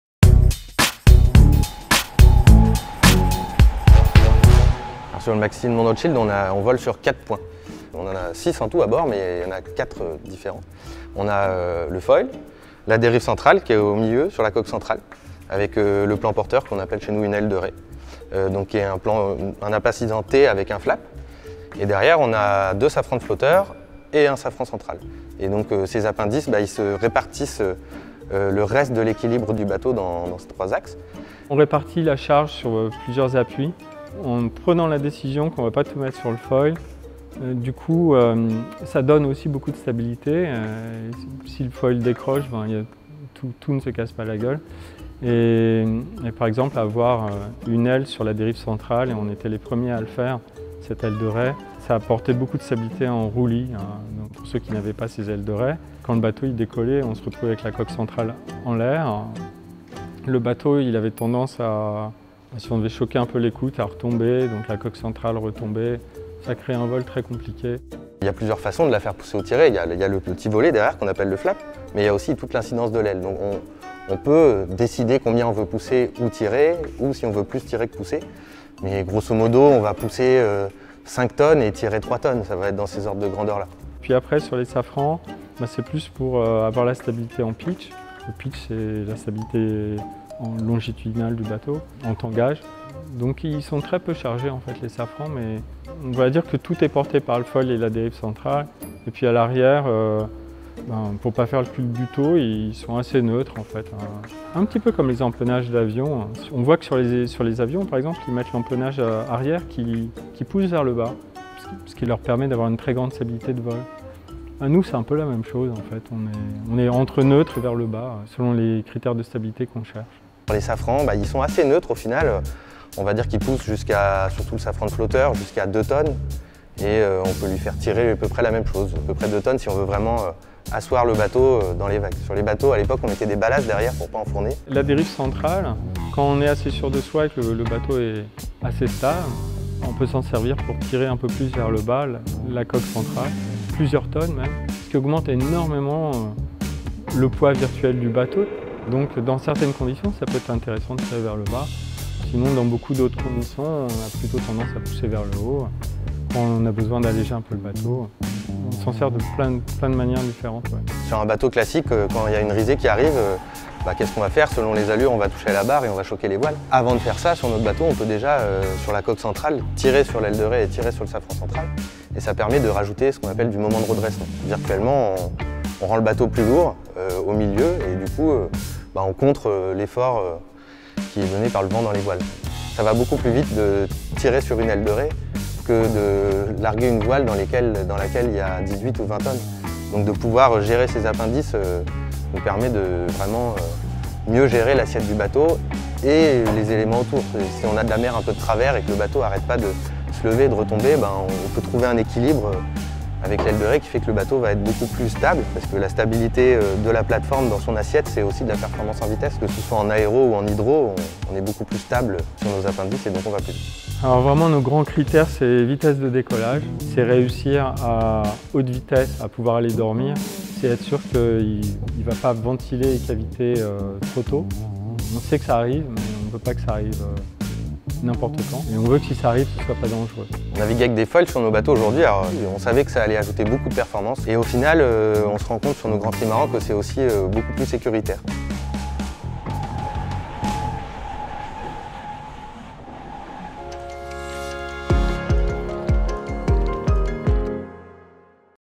The keys to understanding #5: Use of the appendages
💡 Having dived into the history of the foils and explained how they operate, Guillaume Verdier and Sébastien Sainson outline for you how the appendages are used on the Maxi Edmond de Rothschild. Indeed, the appendages enable us to spread the load across the boat and provide the necessary stability and balance for these giants of the seas to fly across the water. Skate wing, rudders, foils… discover how each of these elements work and what they’re used for.

🎥 polaRYSE / Gitana S.A.